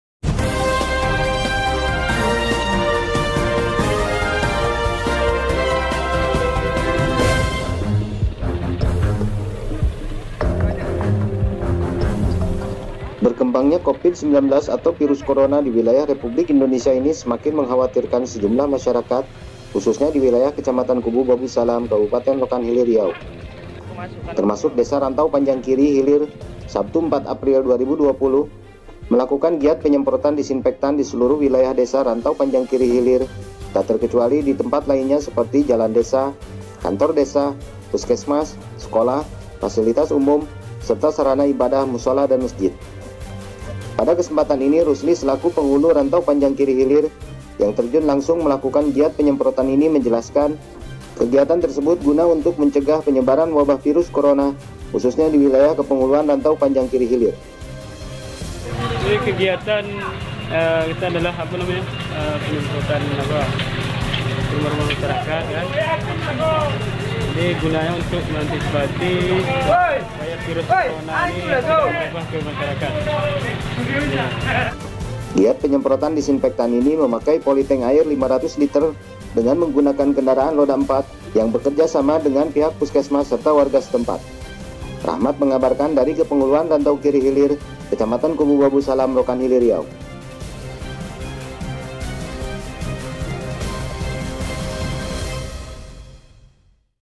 Cegah Berkembangnya Covid-19
Pemkep Rantau Panjang Kiri Hilir Lakukan Penyemprotan